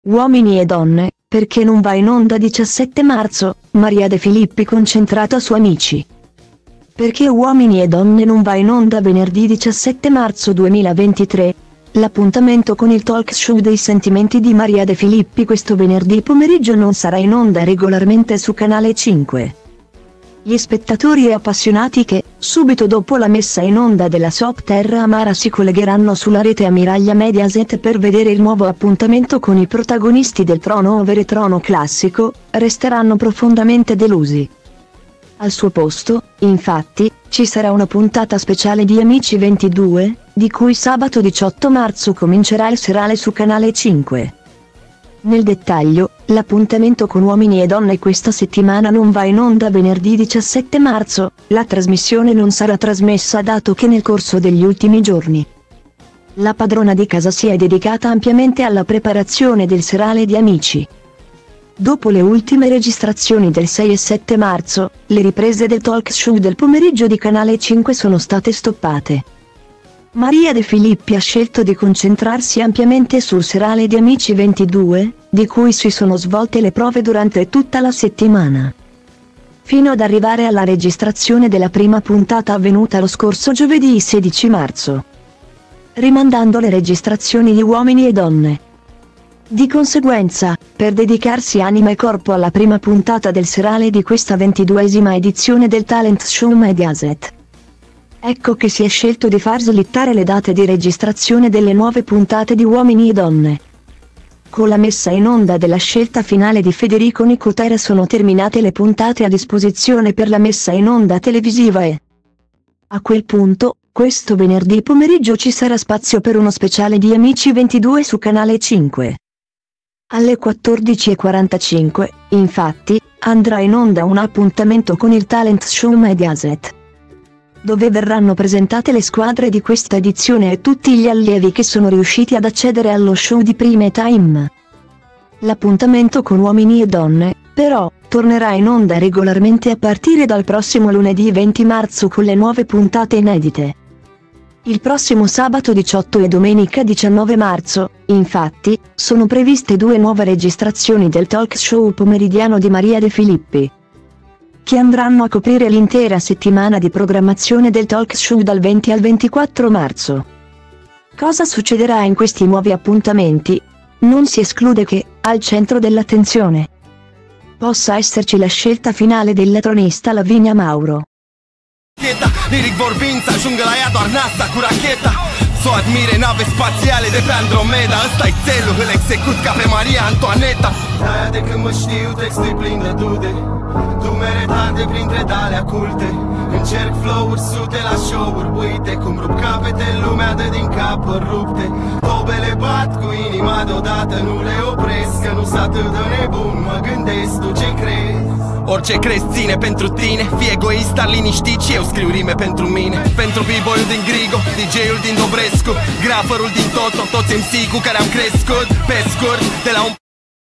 Uomini e donne, perché non va in onda 17 marzo: Maria De Filippi concentrata su Amici
Perché Uomini e donne non va in onda venerdì 17 marzo 2023? L'appuntamento con il talk show dei sentimenti di Maria De Filippi questo venerdì pomeriggio non sarà in onda regolarmente su Canale 5. Gli spettatori e appassionati che, subito dopo la messa in onda della soap Terra amara si collegheranno sulla rete ammiraglia Mediaset per vedere il nuovo appuntamento con i protagonisti del trono over e trono classico, resteranno profondamente delusi. Al suo posto, infatti, ci sarà una puntata speciale di Amici 22, di cui sabato 18 marzo comincerà il serale su Canale 5.